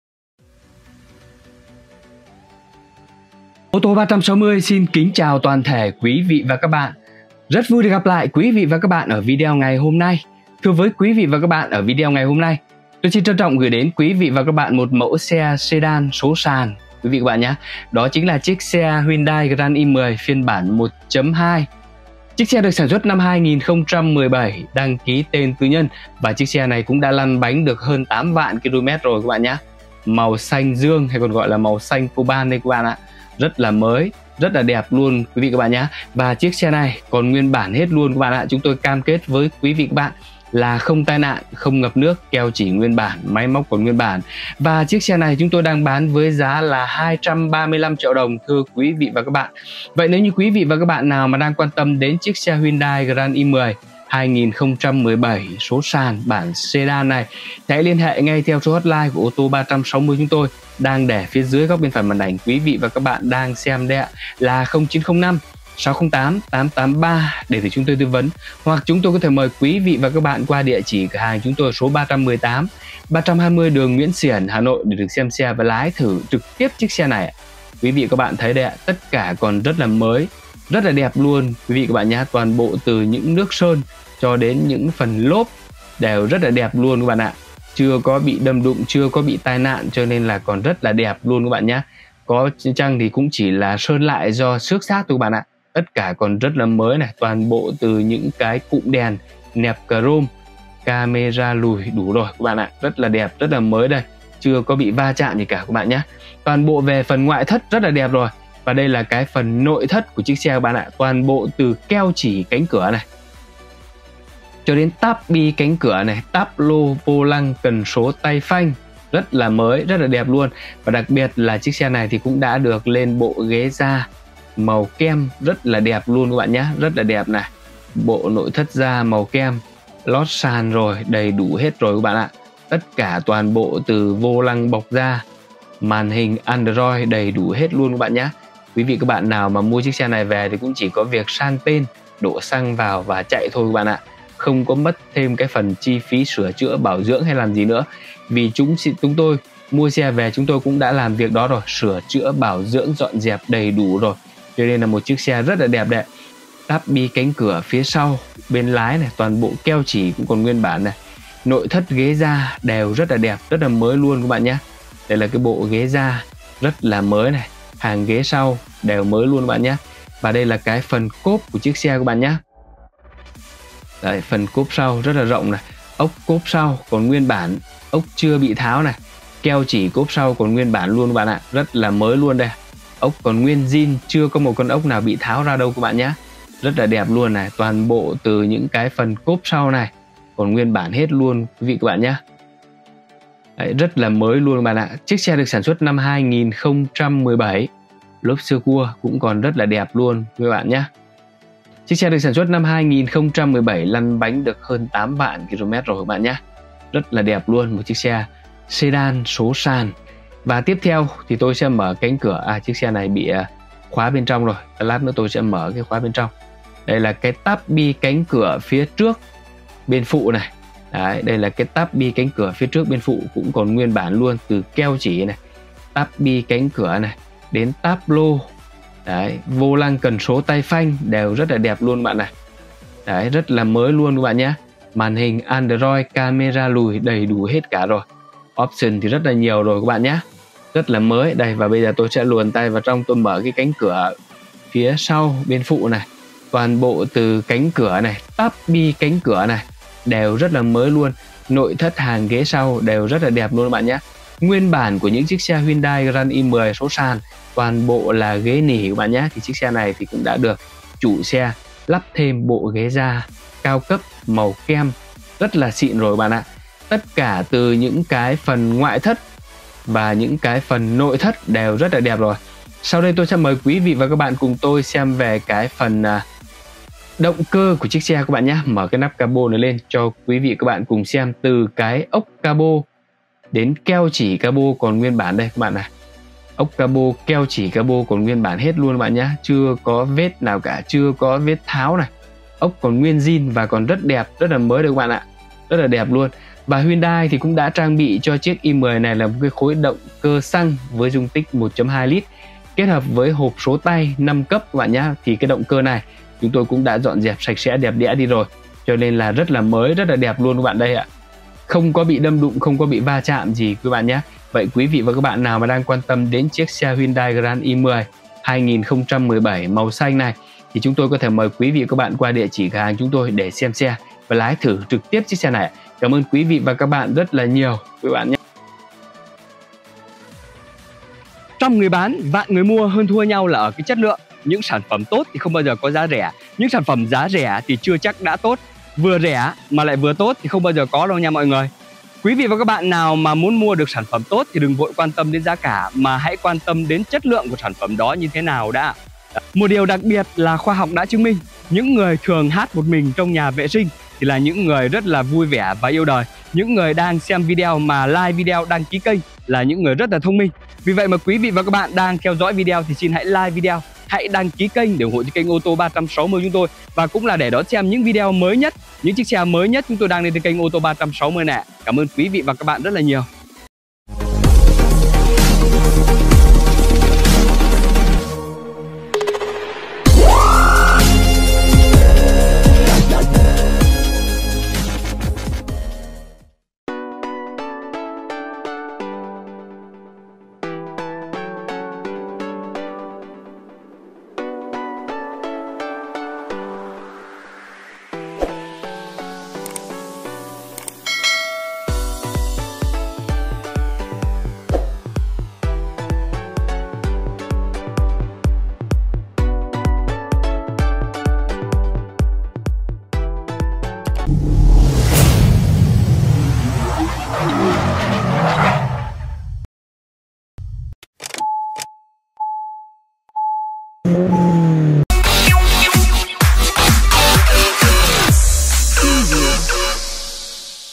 Bán xe Huyndai Grand i10 1.2 sedan 2017 màu xanh dương, màu không sợ đụng hàng với xe Taxi, dịch vụ
Bán xe Huyndai Grand i10 1.2 bản sedan số sàn sản xuất năm 2017, đăng ký tên tư nhân, chạy rất ít, mới lăn bánh hơn 8 vạn km.
Xe được trang bị động cơ xăng 1.2 L hộp số sàn 5 cấp, xe được trang bị nội thất da, vô lăng bọc da tích hợp các phím điều khiển, khóa thông minh. đề nổ Stá/Stop, Màn hình thông minh Android tích hợp camera lùi, cảm biến lùi đầy đủ, gương chiếu hậu gập điện tích hợp xi nhan trên gương...và rất nhiều chức năng khác.
Xe còn rất mới.bảo dưỡng định kỳ tại hãng. cam kết không đâm đụng, ko va chạm, ko ngập nước, máy móc nguyên bản, nội thất cũng rất mới.